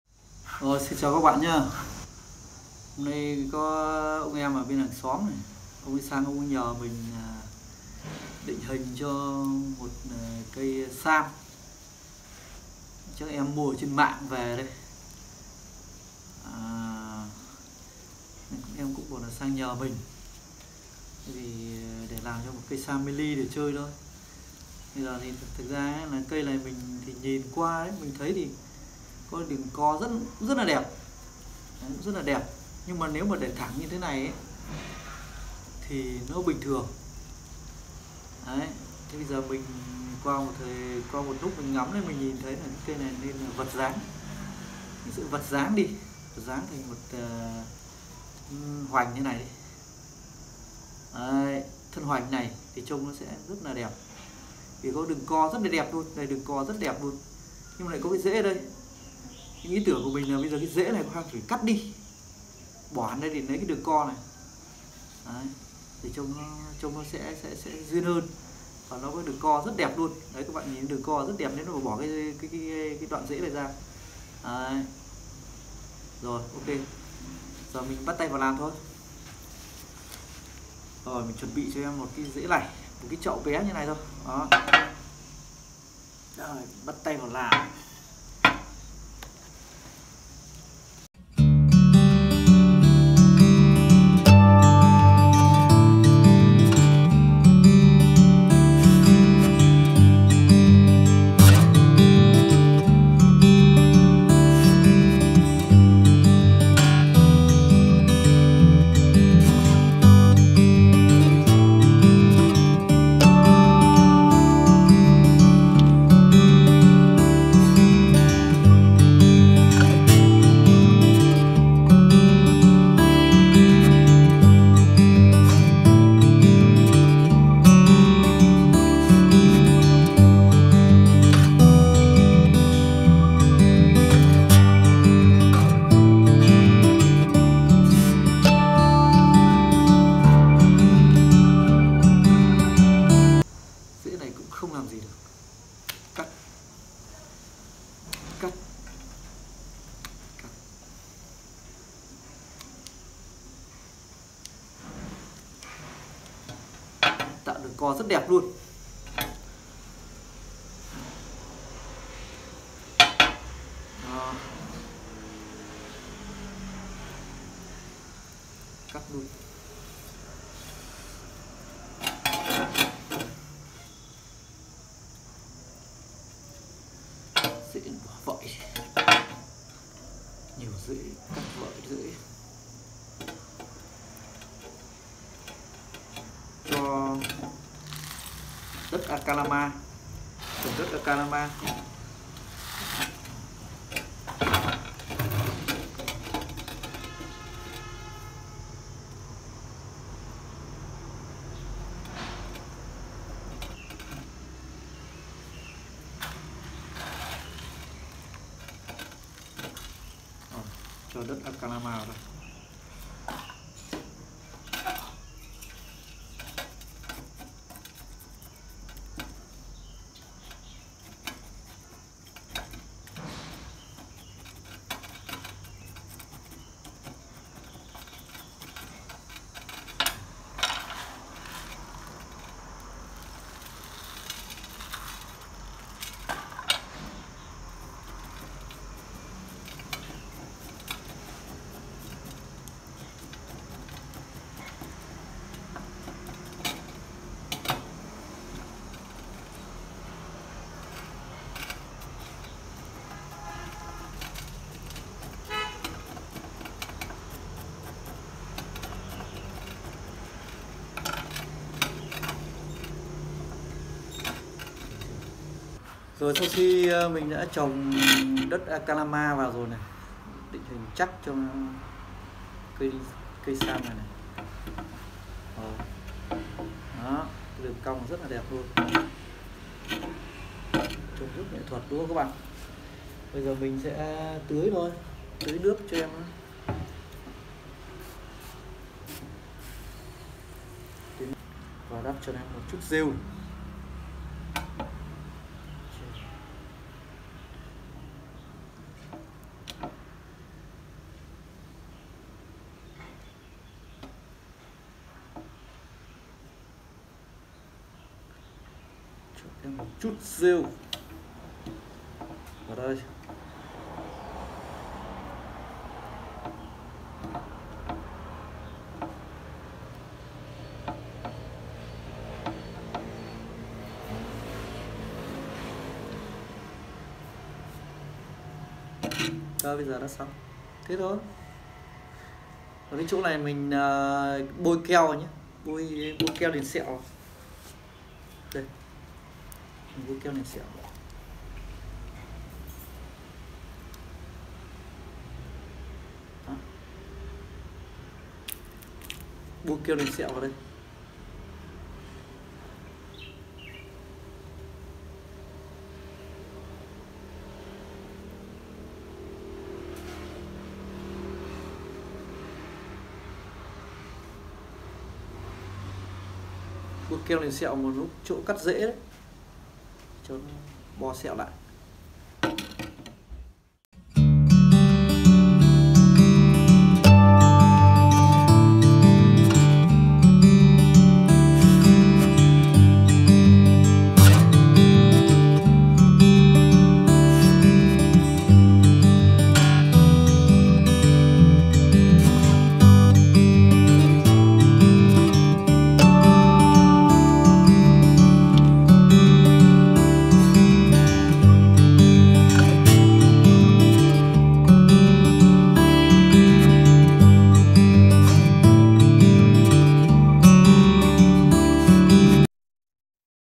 Mr Truyen / Thay Chậu Chuyển Dáng Cho Cây Sam Hương Rất Nghệ Thuật
Thay Chậu Chuyển Dáng Cho Cây Sam Hương Rất Nghệ Thuật